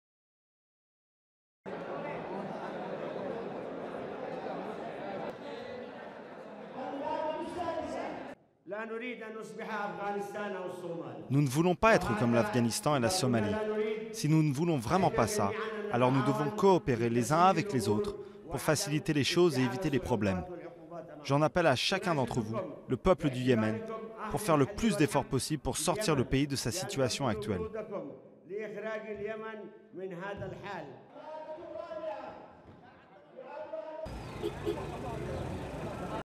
Yémen: le président Ali Abdallah Saleh obtient l'immunité
Le Parlement yéménite a accordé samedi 21 janvier l'immunité au président Ali Abdallah Saleh et entériné la candidature du vice-président Abd Rabbo Mansour Hadi à l'élection présidentielle, prévue dans un mois, dans ce pays secoué depuis un an par des troubles ayant fait des centaines de morts. Images et déclarations. Durée: 00:36.